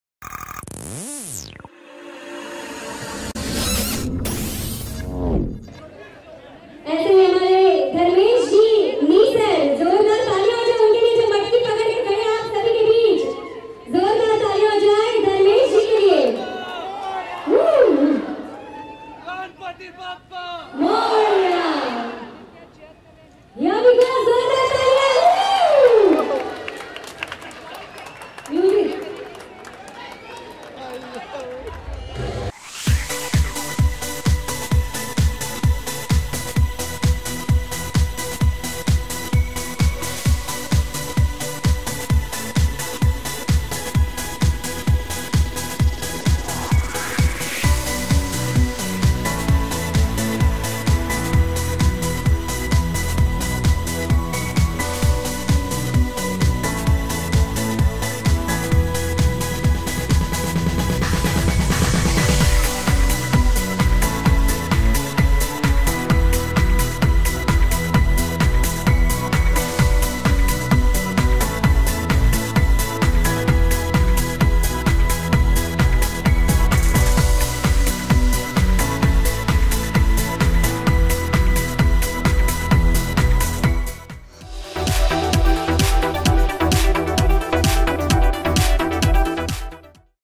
Ranbir Kapoor Breaks Dahi Handi with Fans in Mumbai
Jagga Jasoos actor Ranbir Kapoor was recently seen celebrating Janmashtami with his fans when he broke the Dahi Handi in Mumbai